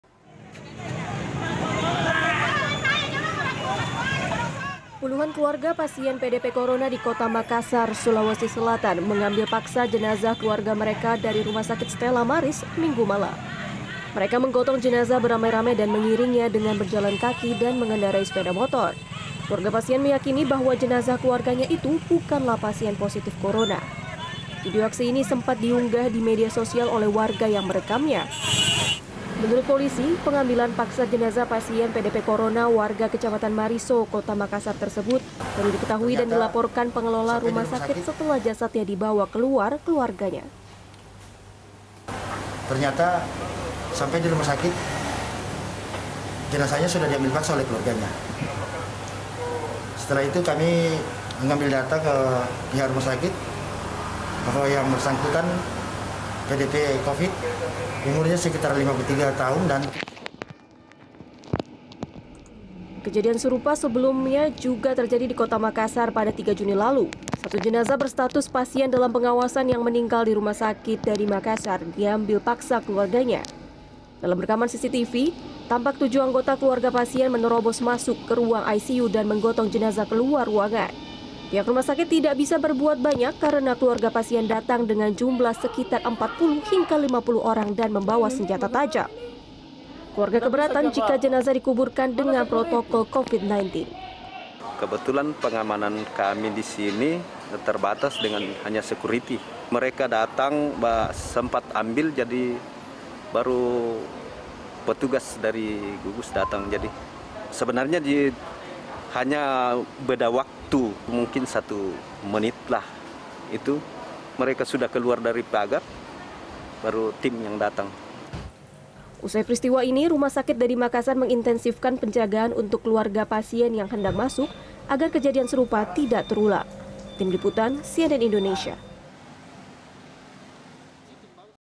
Keluarga Ambil Paksa Jenazah PDP Covid-19
Pasien dalam pengawasan yang meninggal di Manado dan jenazahnya diambil paksa keluarga dinyatakan positif Covid 19. Hasil swab test nya baru keluar dan diumumkan oleh satgas percepatan penanganan Covid-19 Manado, Minggu sore.

Like & Follow Facebook:
CNN Indonesia

cnnindonesiatv